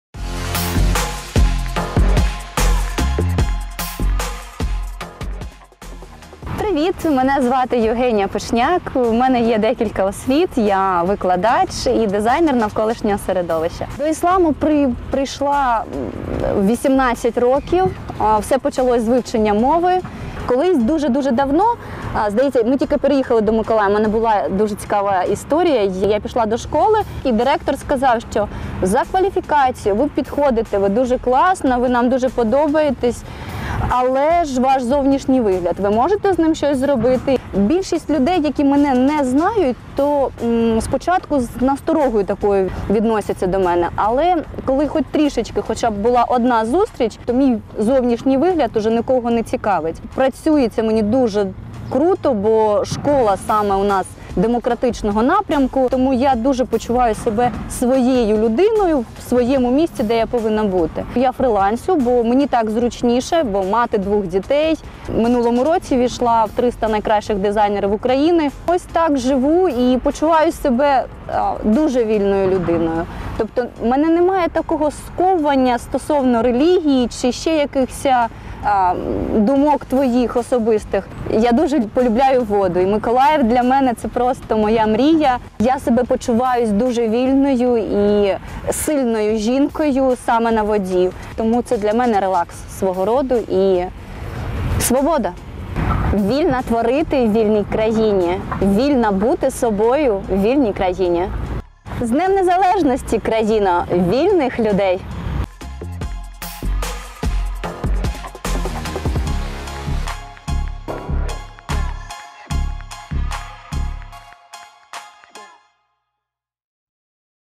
Спецпроєкт Вільні. Євгенія Пишняк
Однією з учасниць спецпроєкту Вільні стала Євгенія Пишняк - миколаївська викладачка предмету технології та дризайнерка-фрилансерка.
У дорослому віці прийняла іслам. Не завжди зустрічає розуміння свого вчинку і деякий час їй навіть складно було знайти місце роботи - директор однієї зі шкіл відмовив саме через зовнішній вигляд. Однак на даний час має і улюблену роботу, і можливість для саморозвитку, і навіть незвичне хобі - SUP-серфінг.
Стверджує, що почувається вільною робити те, що прагне, і ці заняття дають їй відчуття свободи.